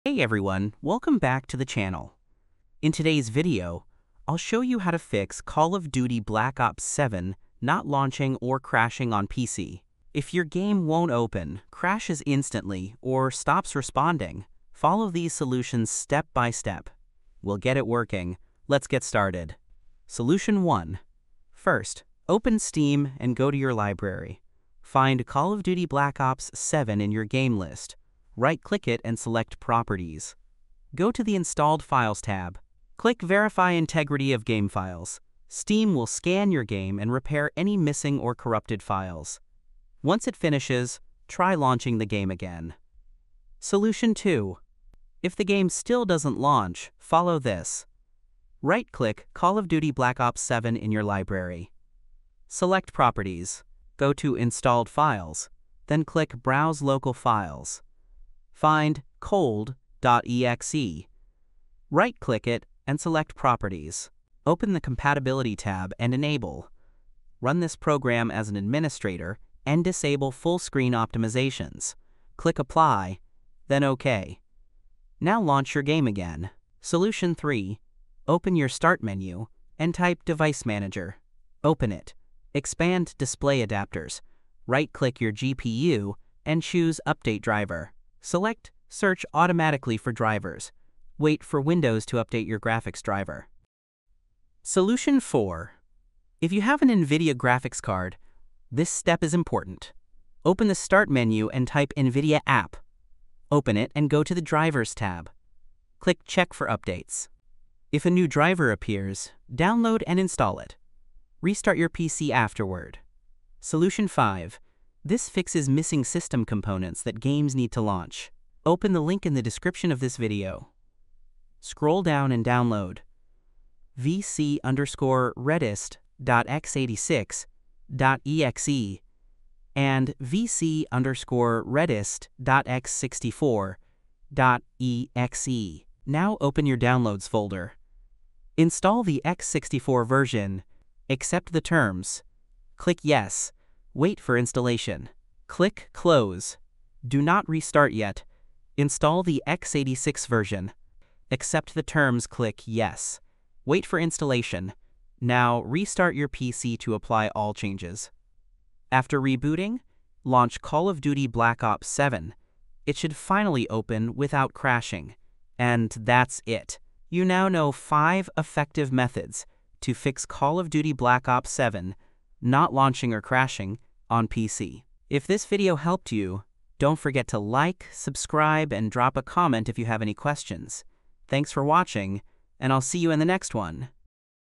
Black Ops 7 Not Launching/Freezing/Crashing/Black Screen/Not Loading/Issue Steam PC FIX🎮🔥
Is Call of Duty Black Ops 7 not launching on Steam?
Does it freeze, crash, get stuck on a black screen, or refuse to load on your PC?

In this video, I’ll show you ALL the best fixes to solve Steam launch issues, missing files, graphics problems, black screen errors, DirectX crashes, and more — so you can finally get into the game and start playing without interruptions.

These methods work on:
Steam (Windows 10 & 11)
NVIDIA, AMD, and Intel GPUs
Both Standard & Deluxe Editions

Chapters:
0:00 - Intro
0:20 - Solution 1
0:43 - Solution 2
1:16 - Solution 3
1:34 - Solution 4
1:53 - Solution 5
2:45 - Outro